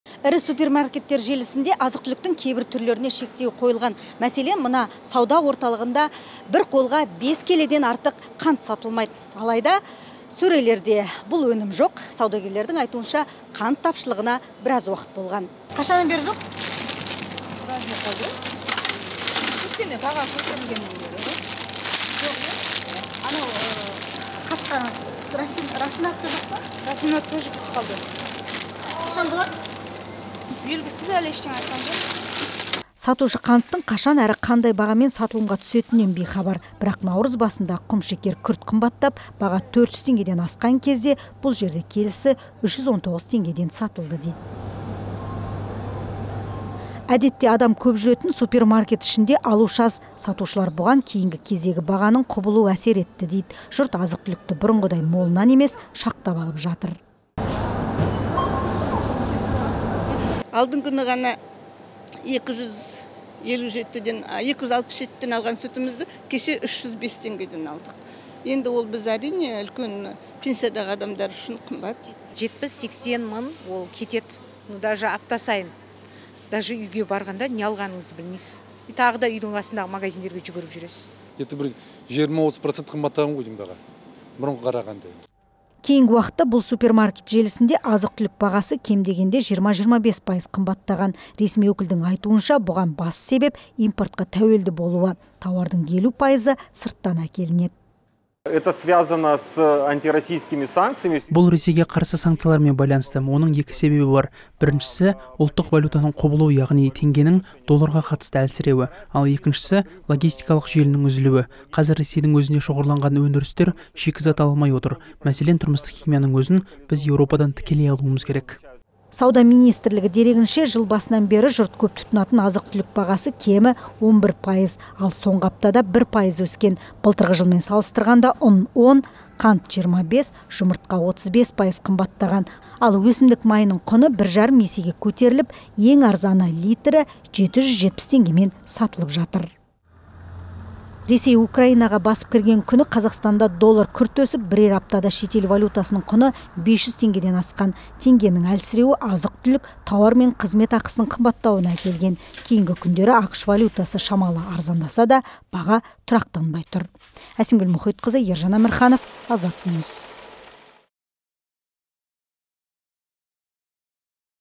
Доллар арзандаса да, азық-түлік қымбаттап барады
Әскері Украинаға басып кірген соң Ресейге Батыстың санкциясы күшейген. Рубльдің күрт құлдырауы Қазақстанға да әсер етті. Теңге әлсіреп, елде доллар бағамы 500 теңгеден асып кеткенде, азық-түлік, тауар бағасы аспандап кетті.